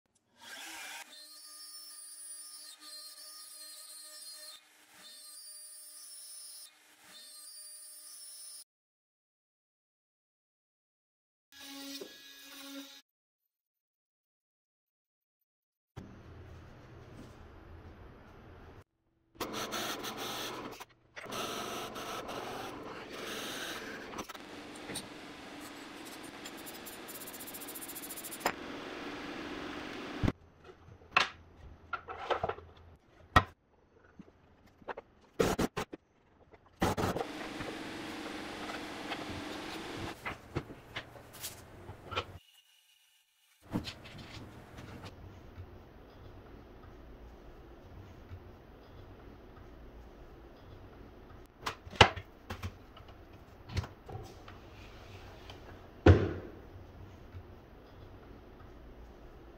Making a CNC Laptop Stand
Making a laptop stand cut on the Shapeoko 4 CNC machine with the Amana Tool item no. 46176-K 1/8" compression bit.
RPM: 18,000
Feed Rate: 40ipm
Plunge Rate: 20ipm
Depth per pass: .25"

Tools Used:

Design Software - Vectric V-Carve Pro -
Amana Tool 46176-K CNC SC Spektra Extreme Tool Life Coated Compression Spiral 1/8 D x 13/16 CH x 1/4 SHK x 2-1/2 Inch Long Router Bit